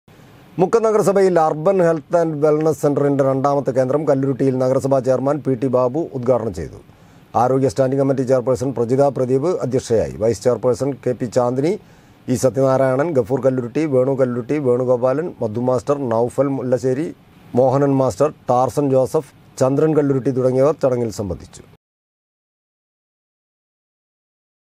മുക്കം നഗരസഭയിൽ അർബൻ ഹെൽത്ത് ആൻ്റ് വെൽനസ് സെൻ്ററിൻ്റെ രണ്ടാമത്തെ കേന്ദ്രം കല്ലുരുട്ടിയിൽ
മുക്കം നഗരസഭയിൽ അർബൻ ഹെൽത്ത് ആൻ്റ് വെൽനസ് സെൻ്ററിൻ്റെ രണ്ടാമത്തെ കേന്ദ്രം കല്ലുരുട്ടിയിൽ നഗരസഭ ചെയർമാൻ പി.ടി. ബാബു ഉൽഘാടനം ചെയ്തു